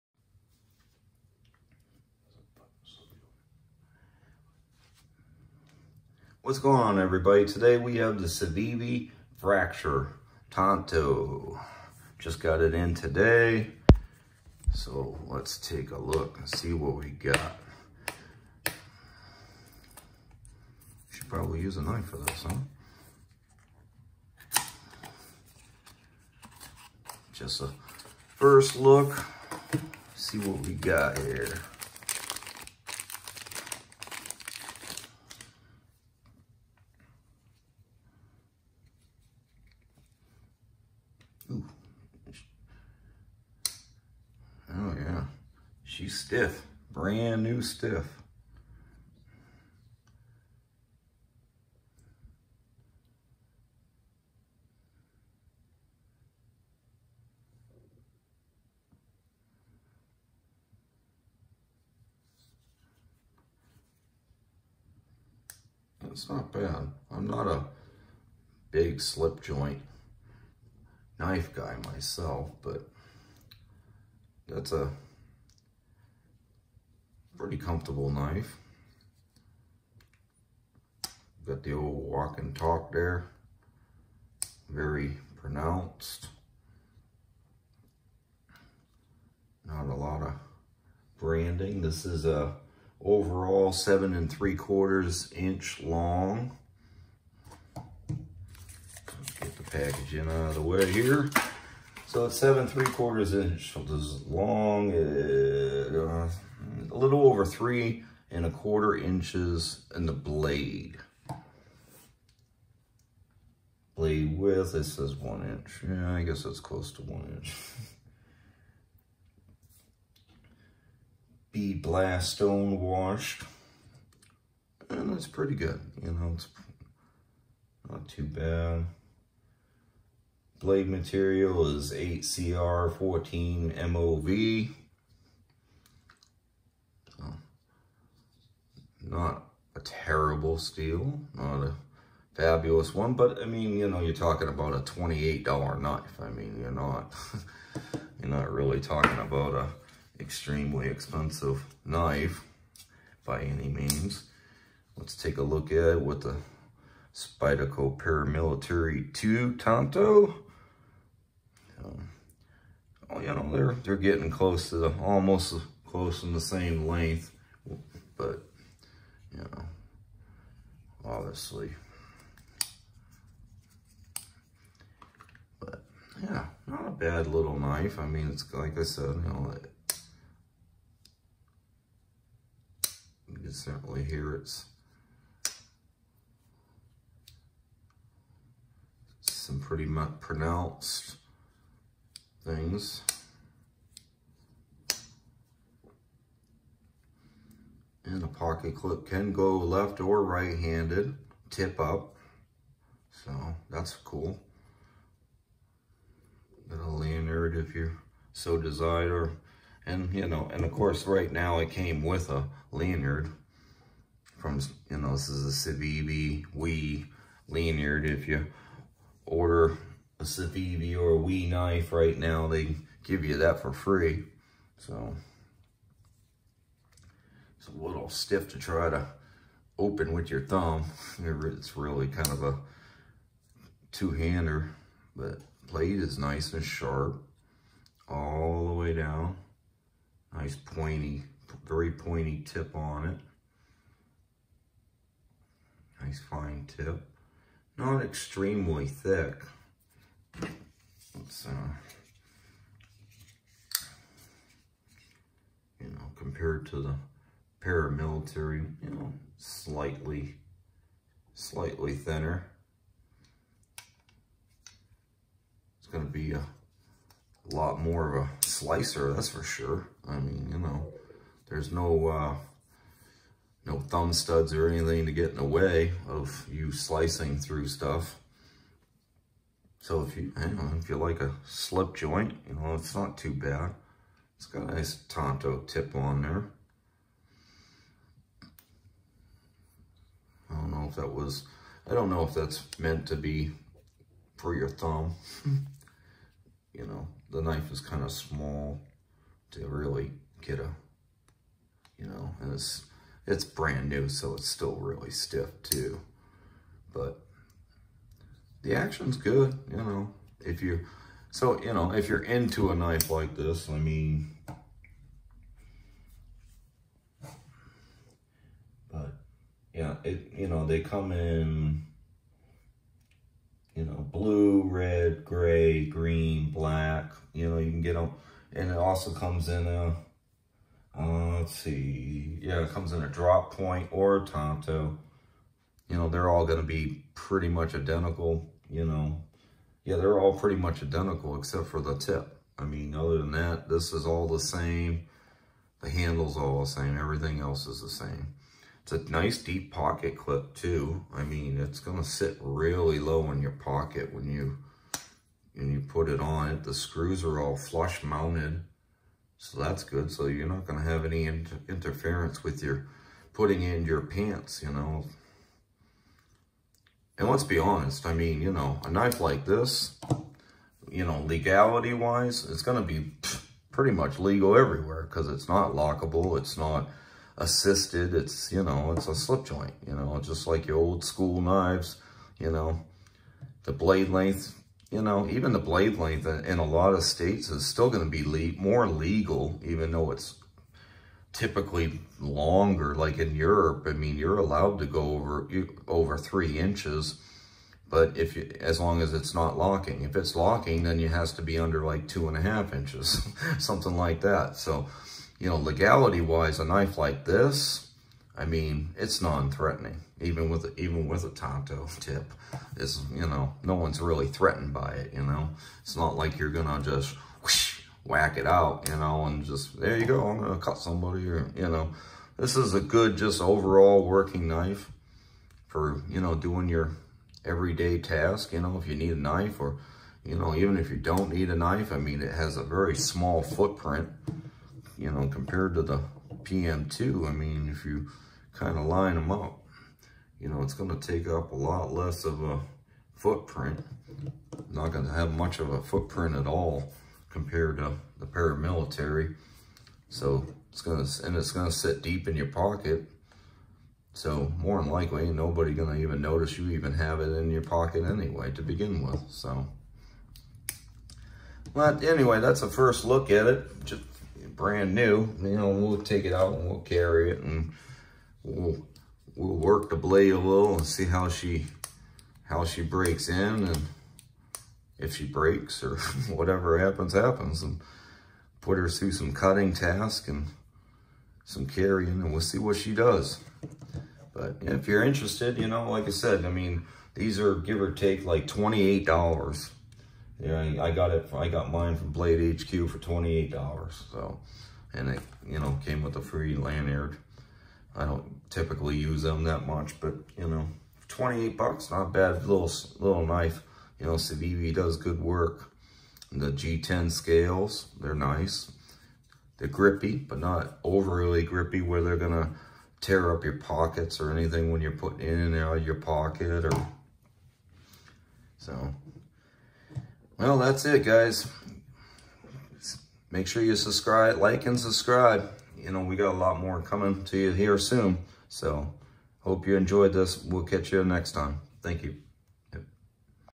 CIVIVI Fracture Tanto Slip Joint Knife
Just Got The New Civivi Fracture model, with blue G-10 handle and a Tanto blade.

The CIVIVI Fracture is a lightweight and handy folding knife. The blade is made from tough 8Cr14MoV stainless steel, and opens using the thumb hole. The Fracture is a slip joint knife, so its legal almost everywhere. The handle is made of textured G-10 with a reversible deep-carry pocket clip, and the back spacer forms a small lanyard hole.